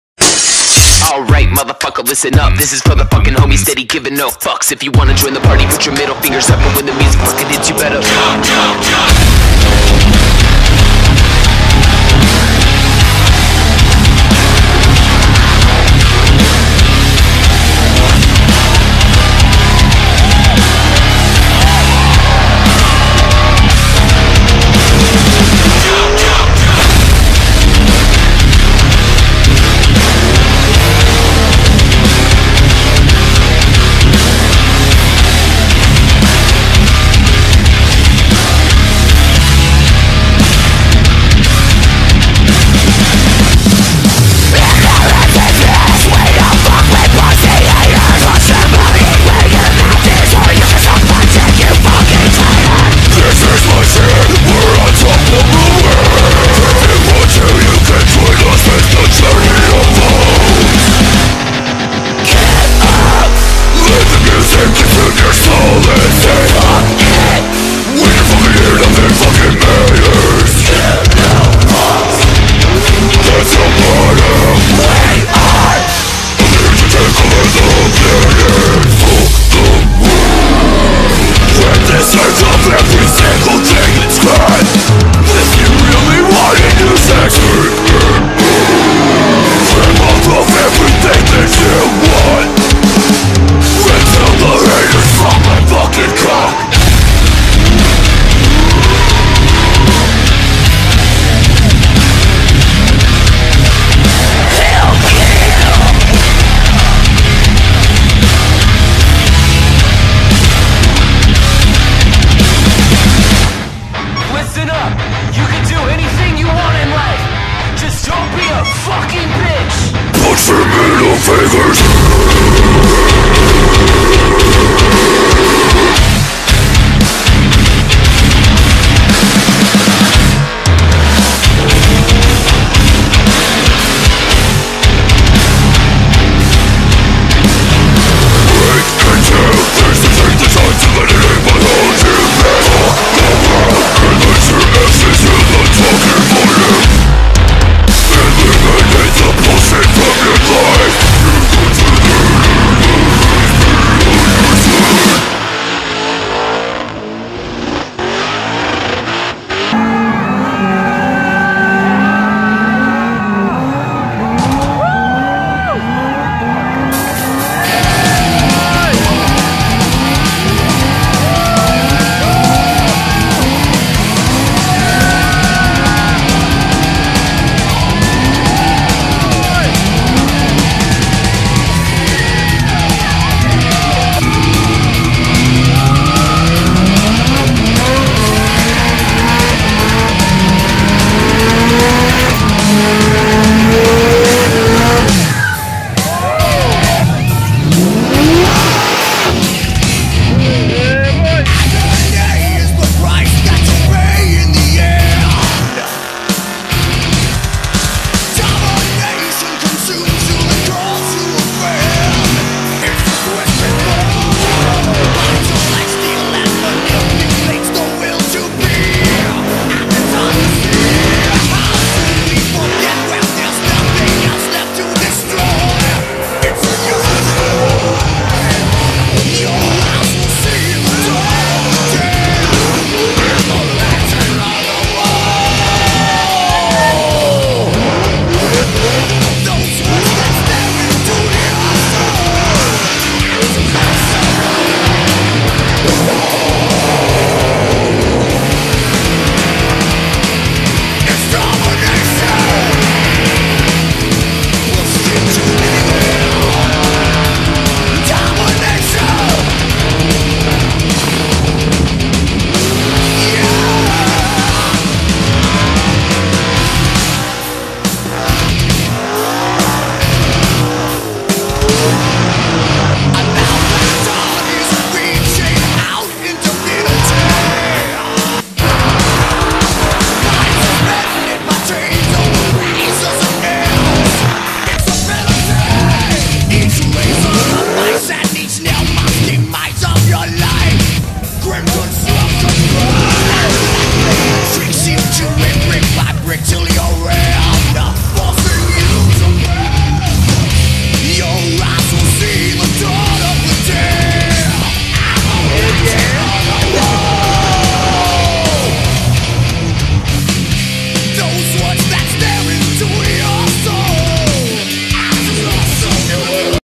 summernats 2017 video edit 31 3 17
Finally got around to editing some of my footage from summernats. Its amateur as but still a vid. ;P